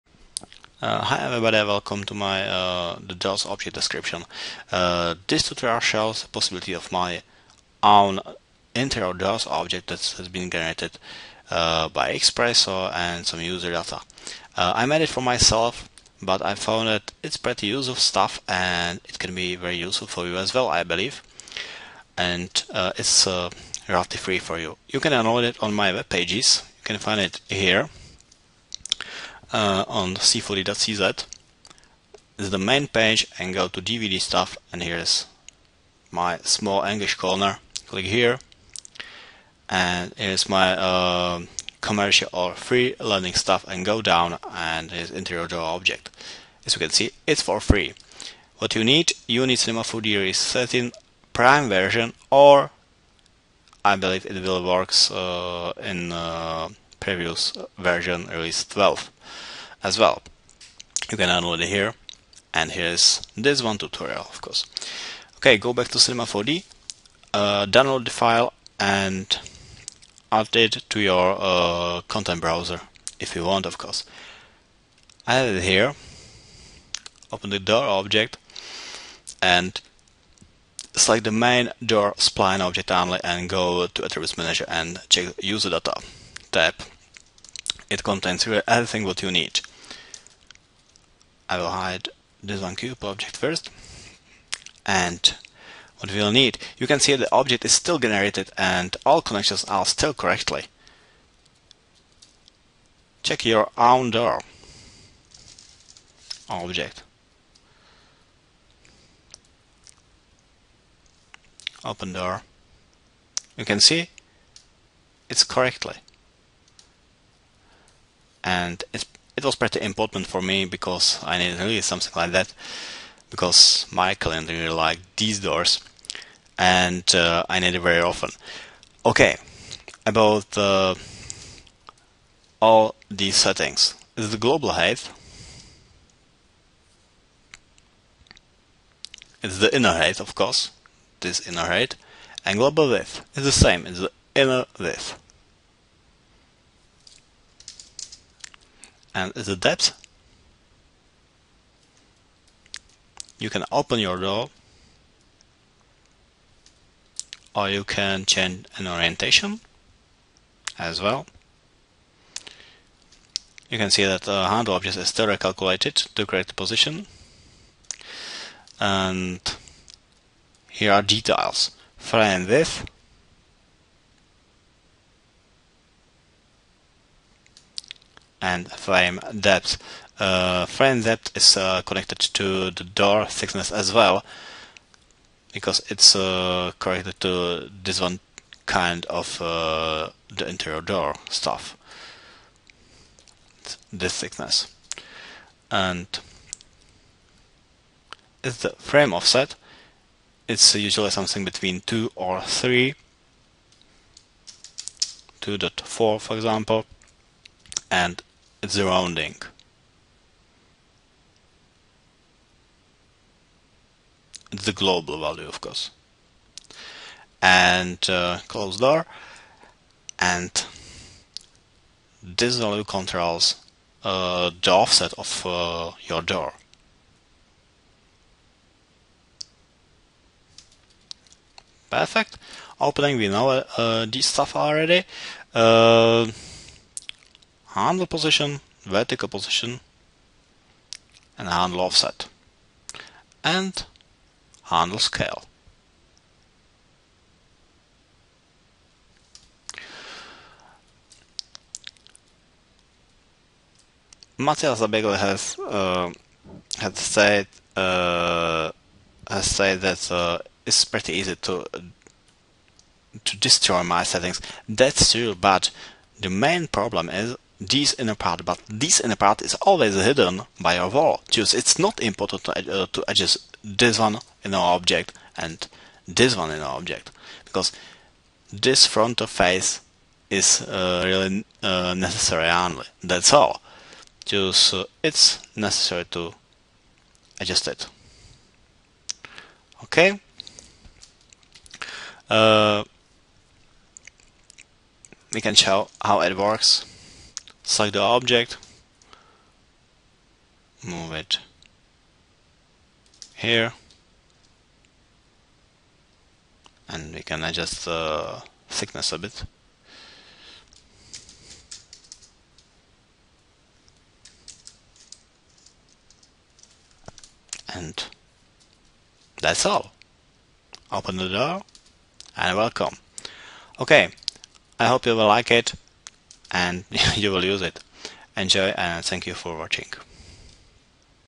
The Door object description
Short tutorial shows how the door object works. The object is available here for free (for CINEMA 4D R12/13)